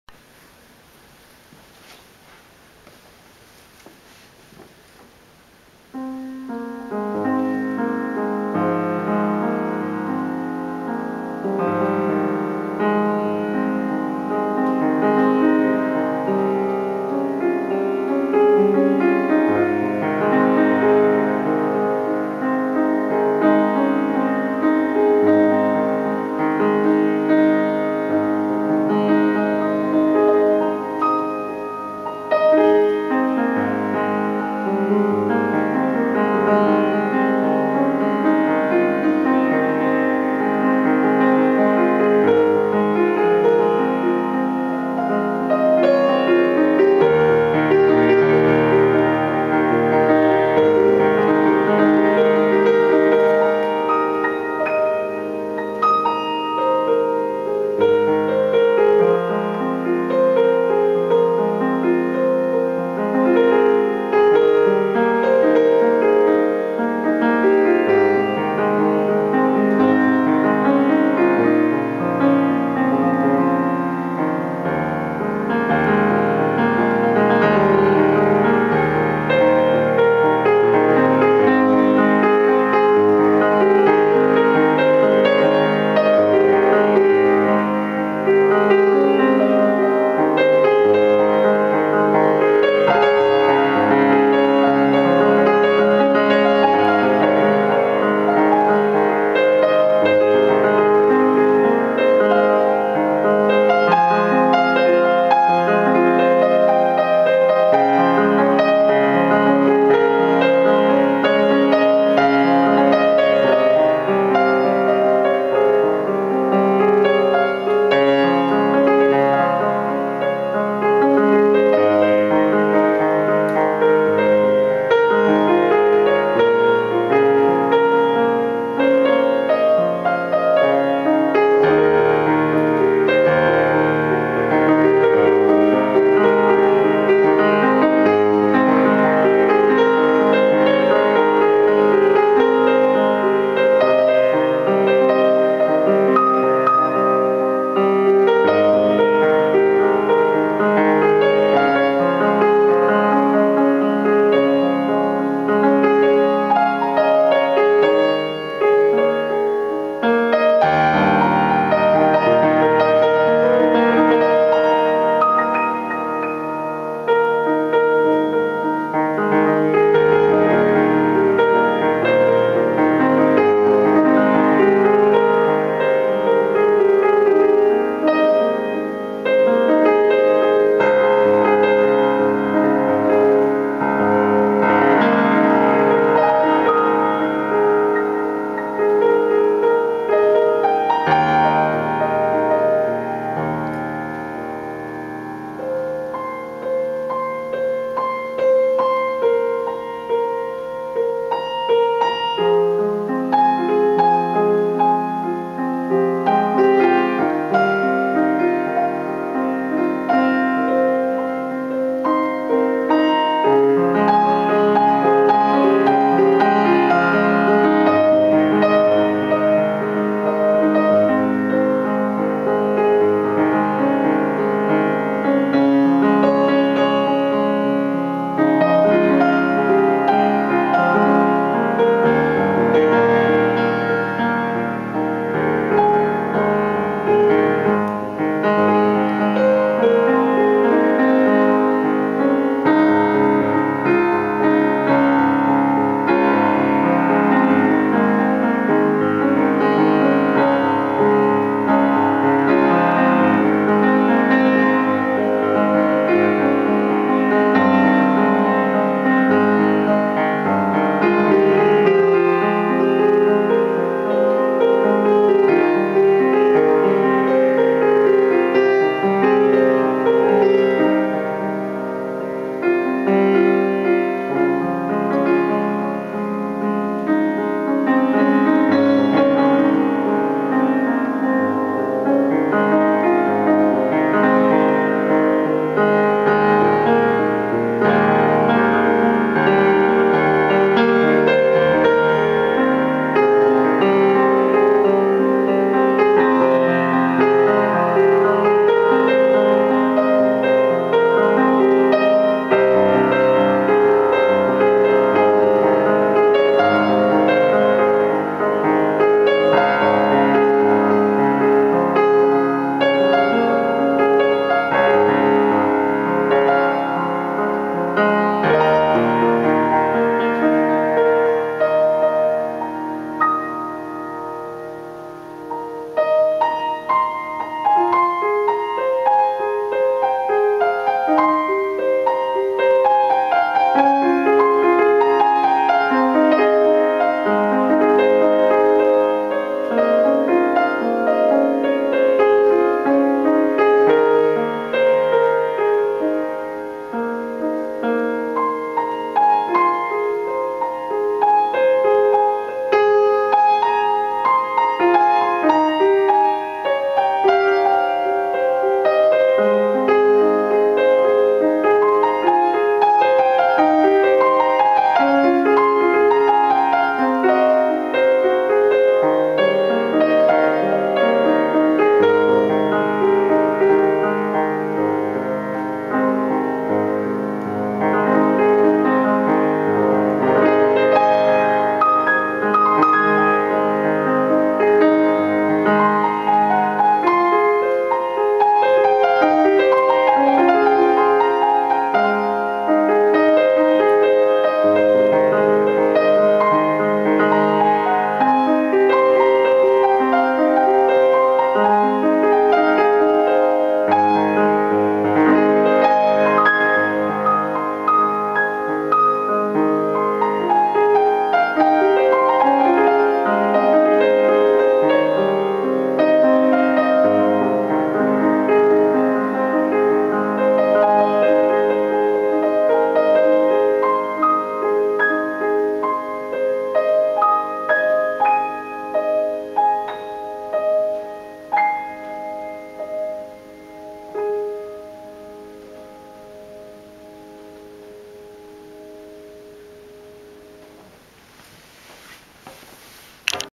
Playing my own song on a college grand piano
Me playing an improvised song on the piano. Hope you enjoy it. Sorry for the bad sound quality.